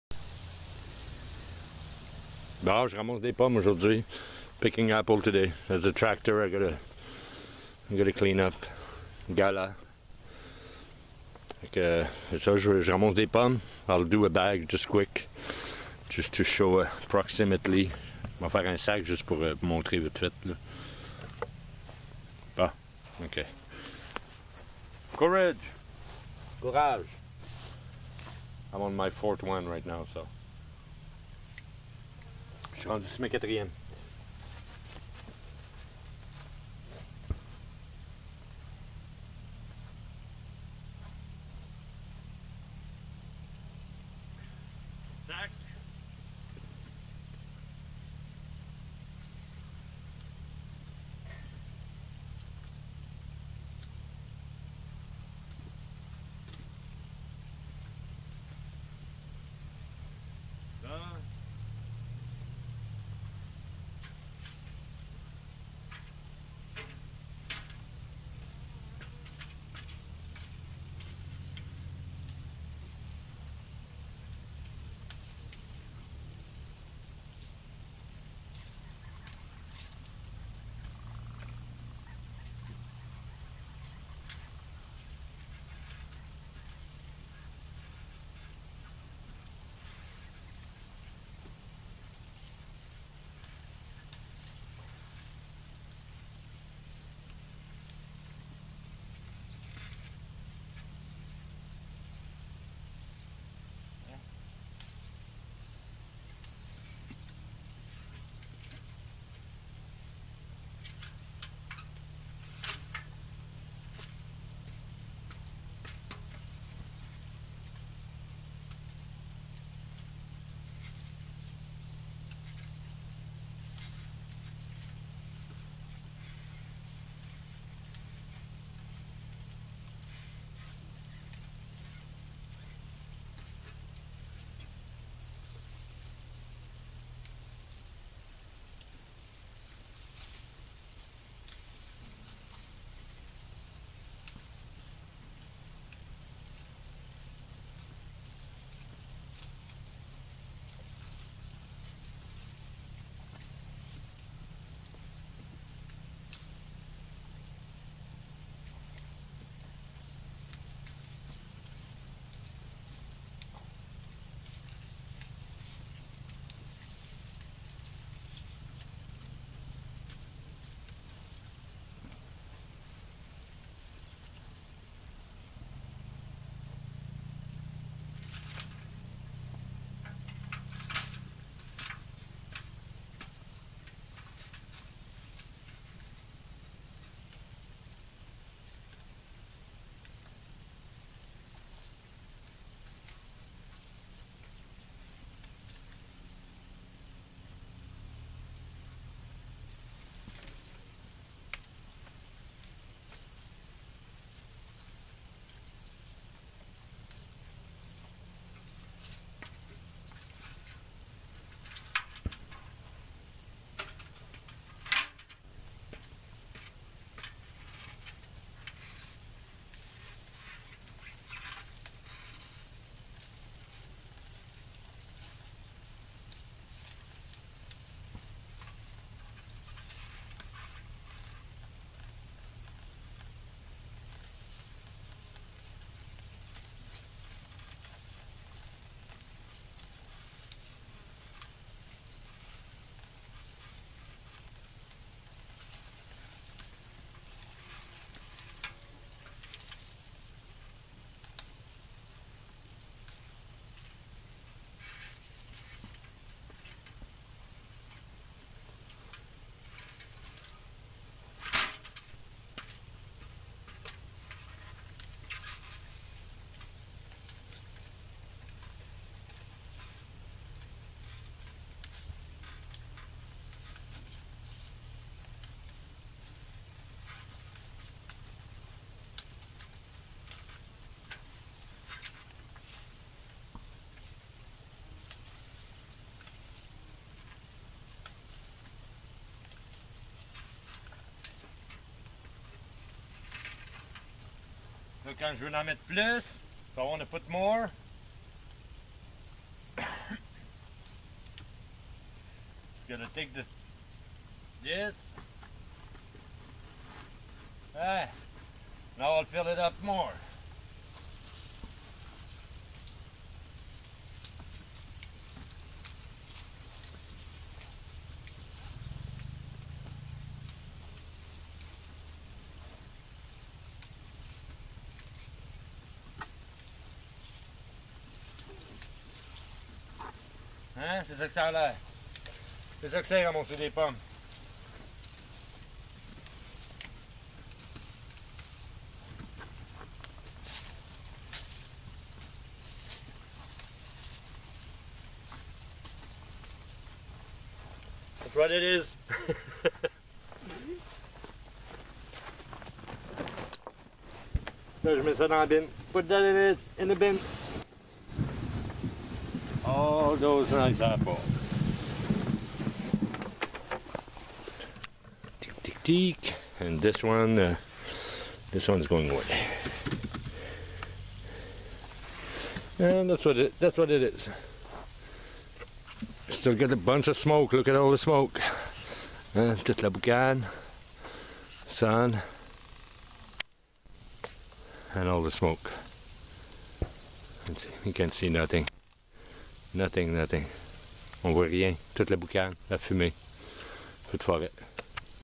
Picking High Density Apples
This job is not for everyone, I carry a few ton's on my shoulder and back and they can't be bruise, farmer will kick you out fast if you damage their only crop of the years, average 6oo pounds each bin more or less depending on size and density, 10 bins is about 6 tons, my record in the 90's is 16 bins sun up to sun down and go go go, thanks.